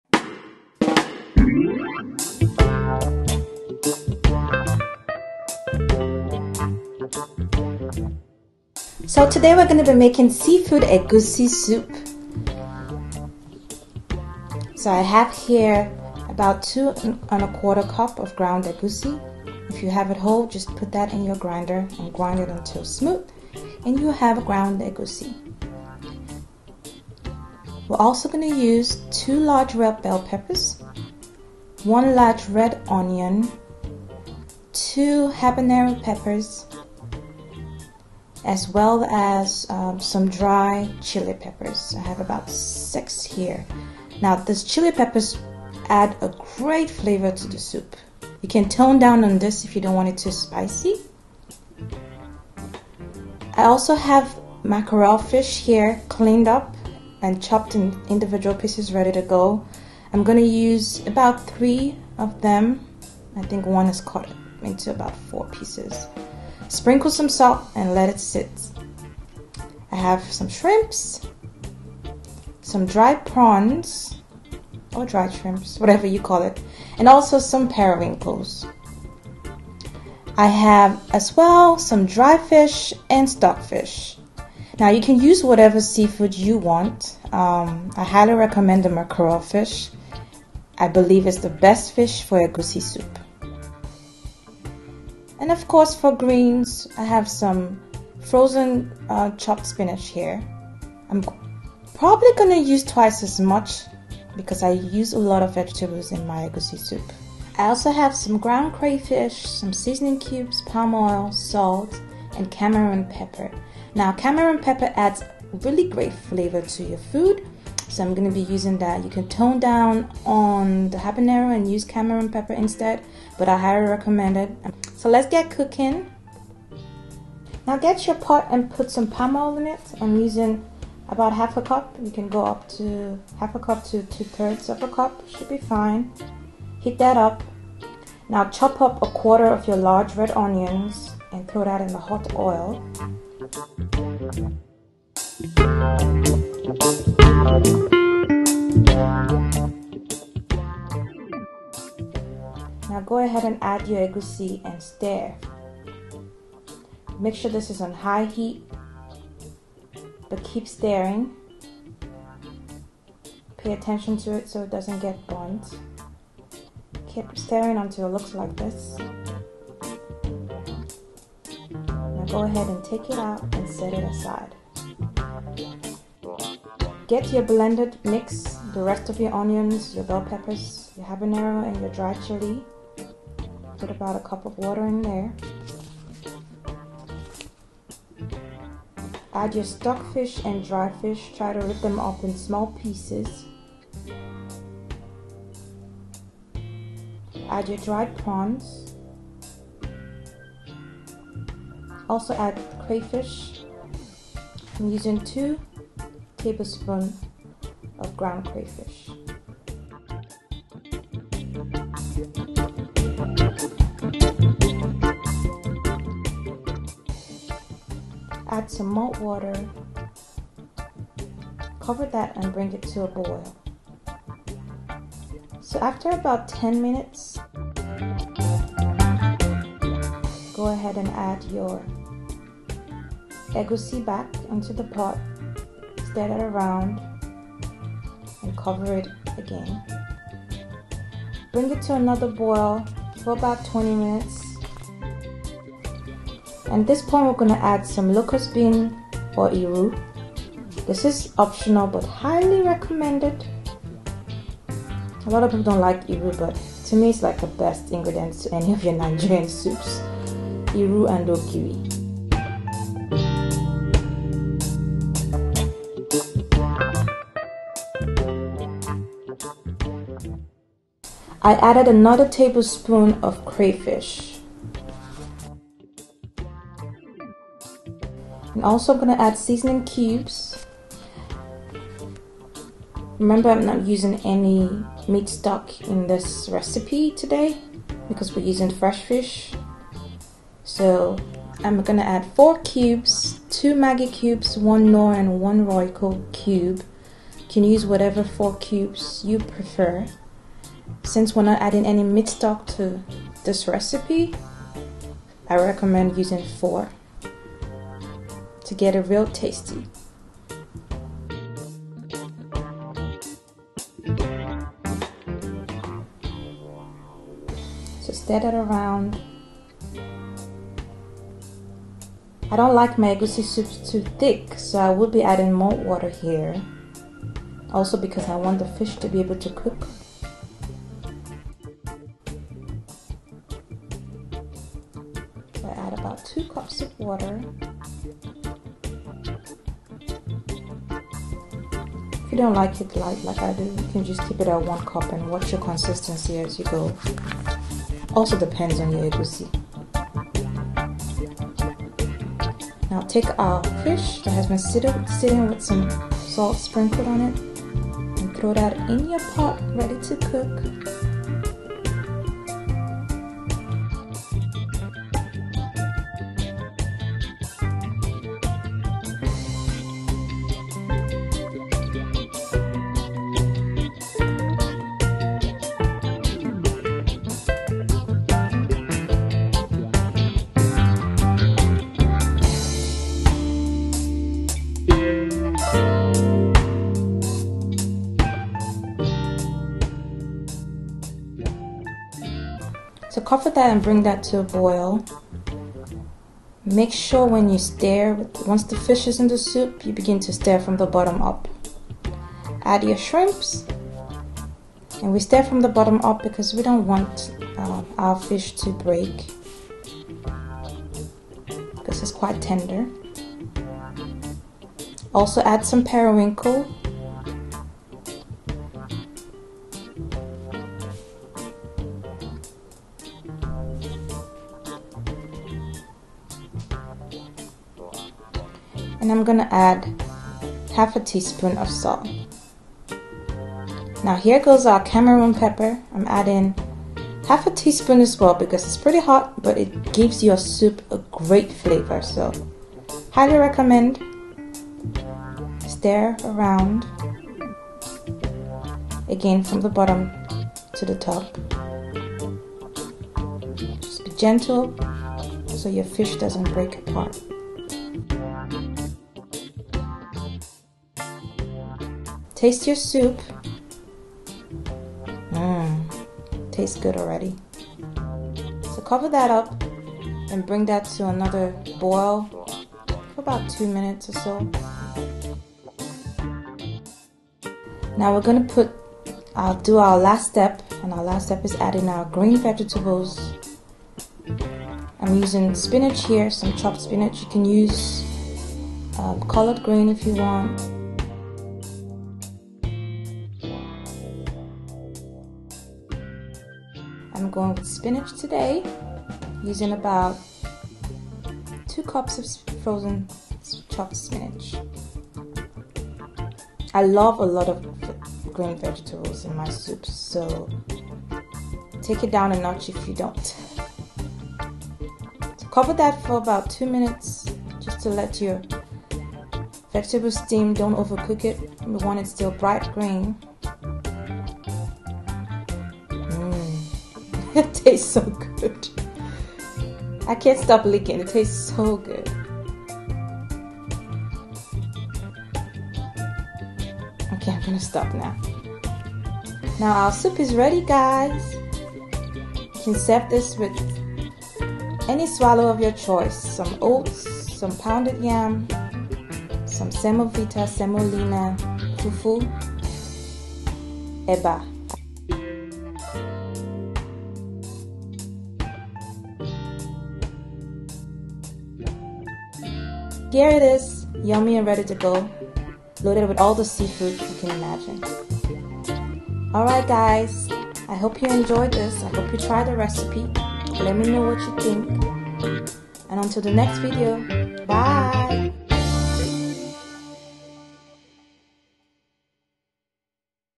How to Make Nigerian Egusi Soup | Seafood Egusi Recipe | Different Method
Egusi Soup also known as Melon Soup is a popular Nigerian delicacy which can be cooked with many wonderful different methods. In this video, I share one of my delicious methods using mackerel fish, shrimps, periwinkle and other ingredients listed below. I call this SEAFOOD EGUSI SOUP.
I hope you try my unique recipe for your next Egusi Soup adventure.

INGREDIENTS / SHOPPING LIST
2 1/4 Cups of Ground Egusi
2 Large Red Bell Peppers
1 Large Red Onion
2 Habanero Peppers
6 Dry Arbol Chili Peppers
3 Mackerel Fish
1 Cup of Shrimps
1/2 Cup of Periwinkle
1/2 Cup of Dried Prawns
1 Cup of Stock Fish
1 Cup of Dry Fish
2/3 Cup of Palm Oil
Seasoning Cubes
Ground Crayfish
Dry Cameroon Pepper
Locust Bean/Iru (Optional)
Salt